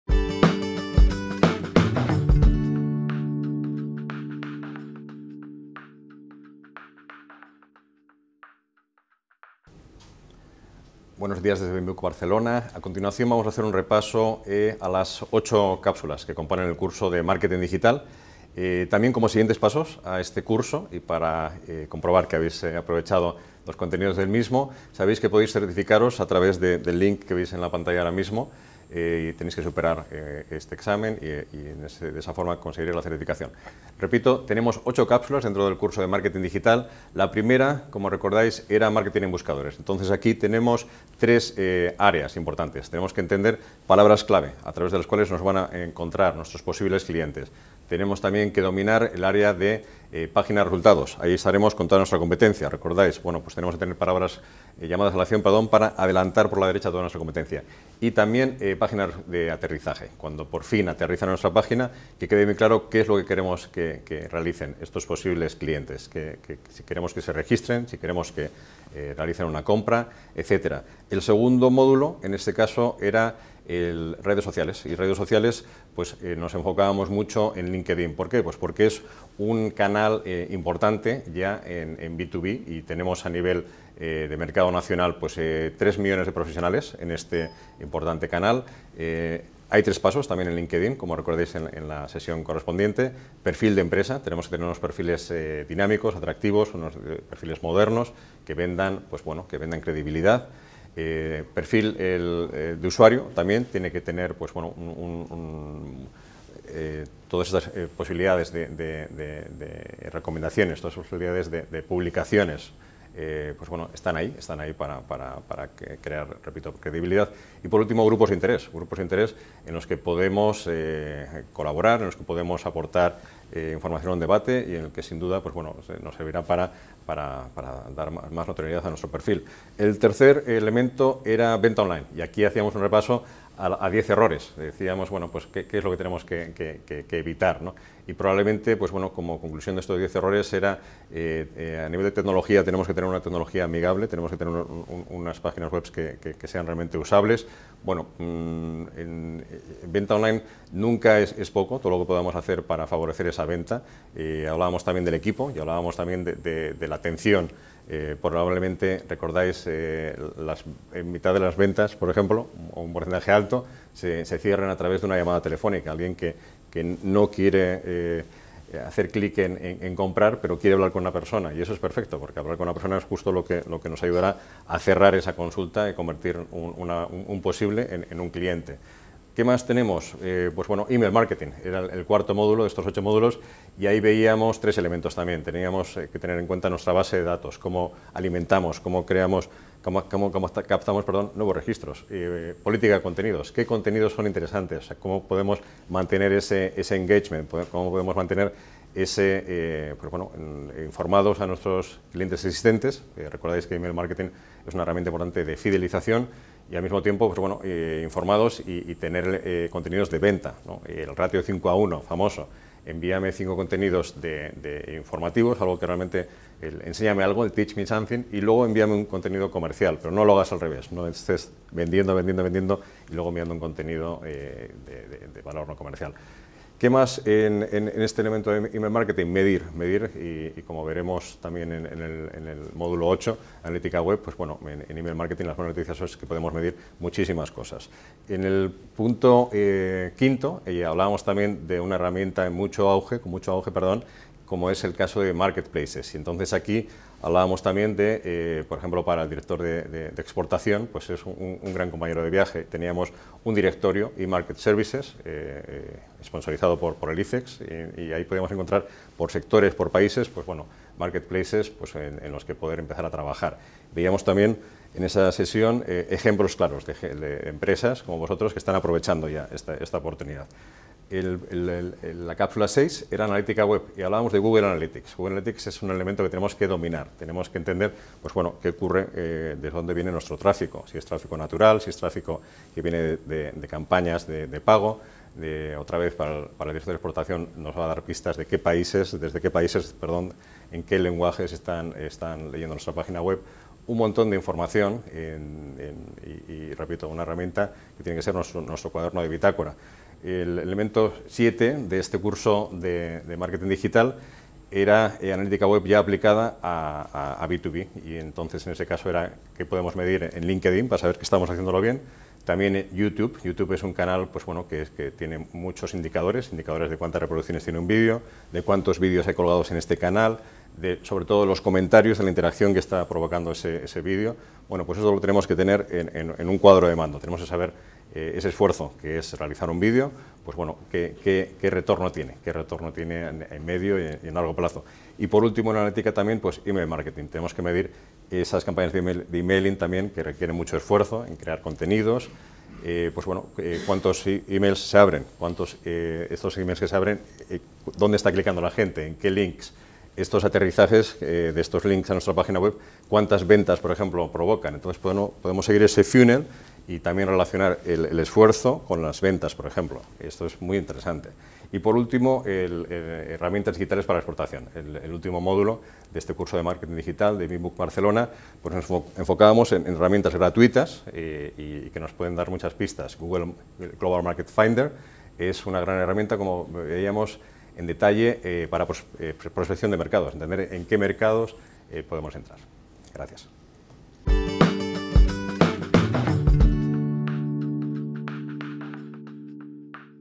9/9 Marketing Digital: 9 pasos clave
"Marketing DIgital" CURSO GRATUITO

Puedes acreditar los conocimientos adquiridos superando el TEST que encontrarás junto con otros cursos de tu interés

Durante estos 9 vídeos de "Marketing digital" vamos a repasar todas las herramientas a tener en cuenta para el desarrollo de nuestro plan de marketing. Desde el análisis de buscadores, pasando por el análisis de las redes sociales profesionales, hasta el análisis de las herramientas de venta online, los market places o el e-mail marketing entre otros. ¡SÍGUENOS!